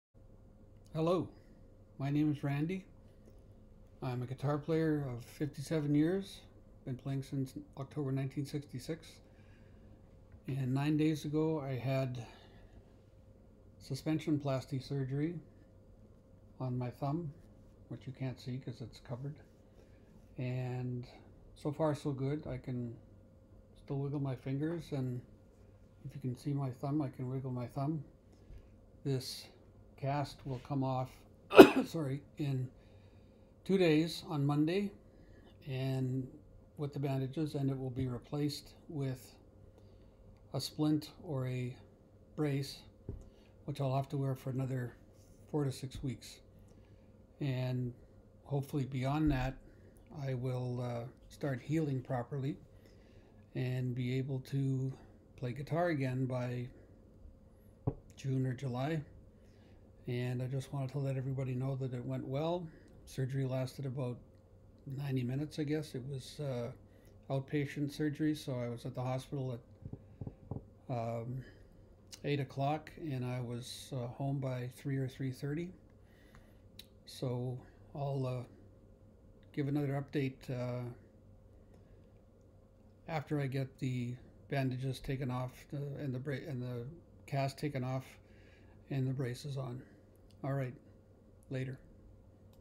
Suspensionplasty Surgery of Left Thumb Basal Joint.
I had suspensionplasty surgery on my left thumb basal joint on 21 March 2024.